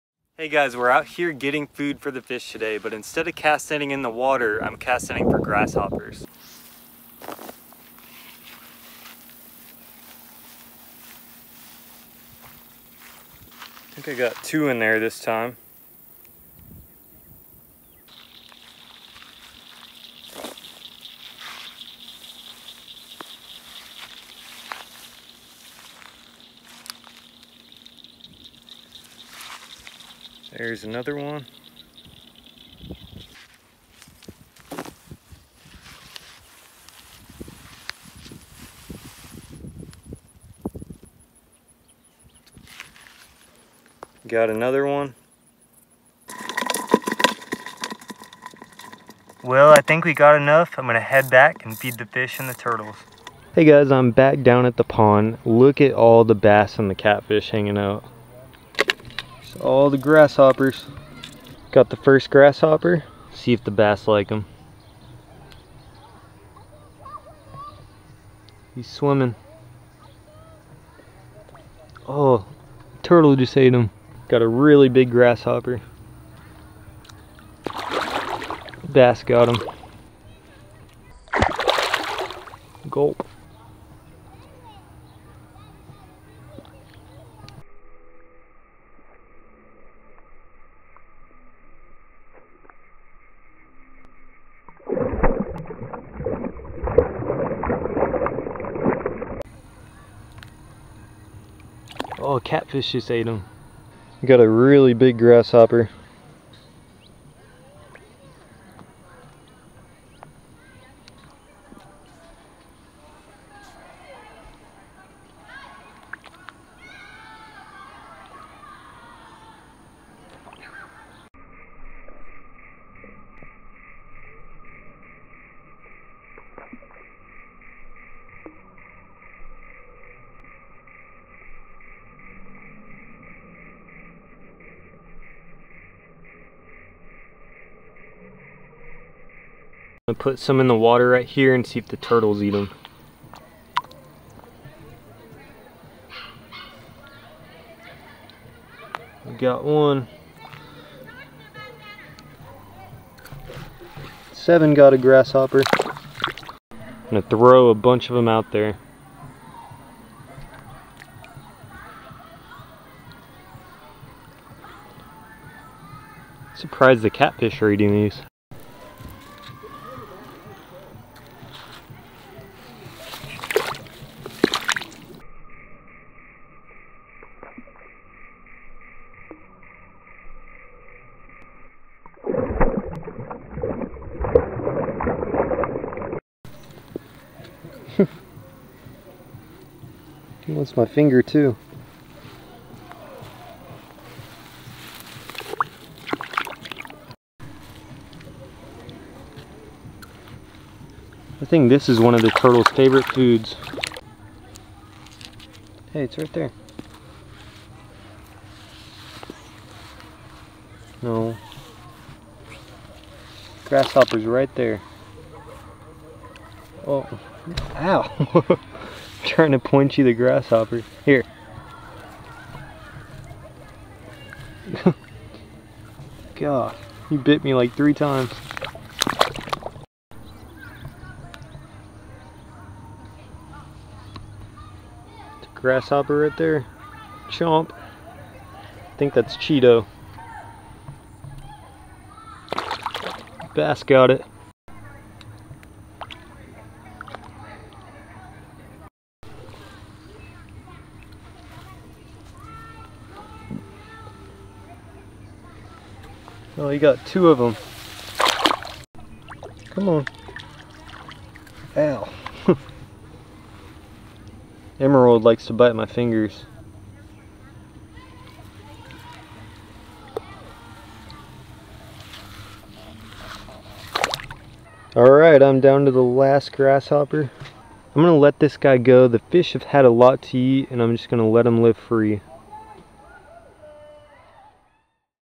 Bass Love Grasshoppers!
In this video, I catch grasshoppers using a cast net to feed the fish and turtles. I caught over 30 grasshoppers and the bass, catfish and turtles loved them. Thank you guys for watching!!!